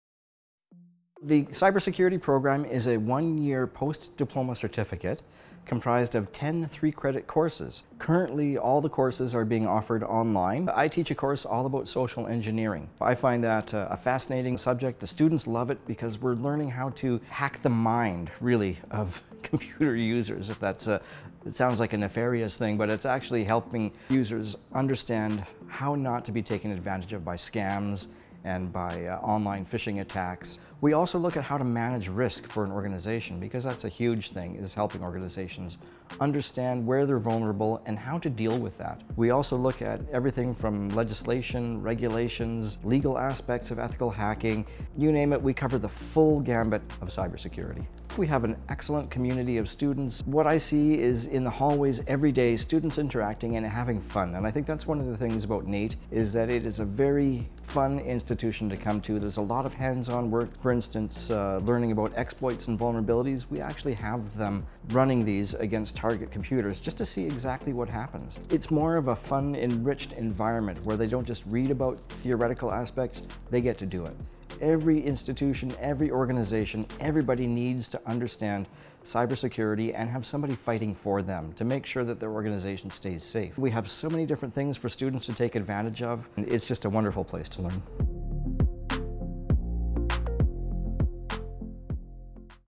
Study Cybersecurity at NAIT
Cybersecurity is essential to protecting organizations, networks, applications and users from a world of online threats. NAIT’s Cybersecurity post-diploma certificate will establish you as a leader with essential skills in a high-demand field.

You will gain advanced operational skills and security strategies to defend against attacks and manage risk. You will also learn about the technical, legal and political issues regarding cybersecurity.